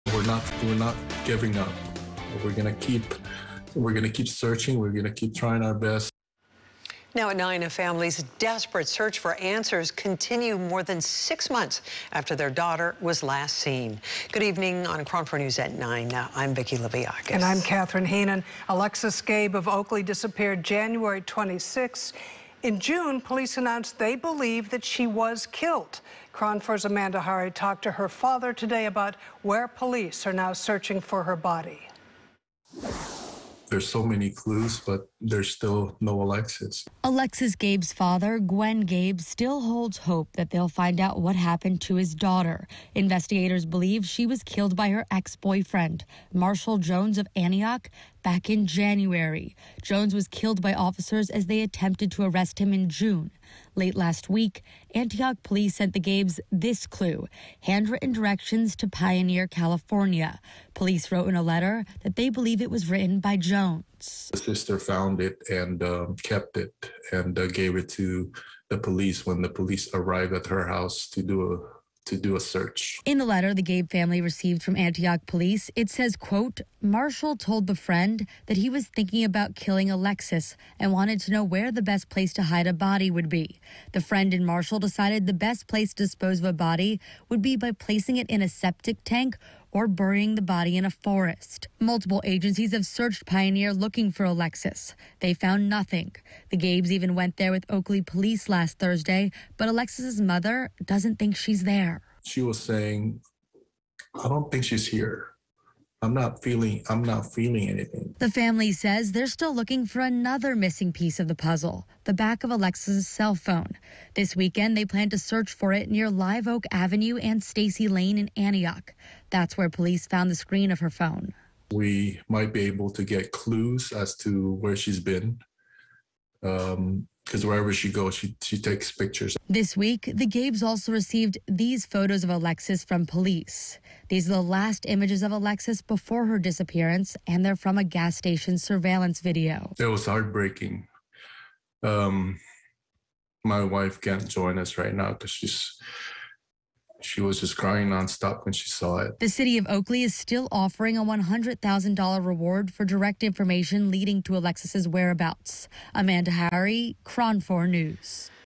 Update on the search for Alexis Gabe
KRON4's Amanda Hari reports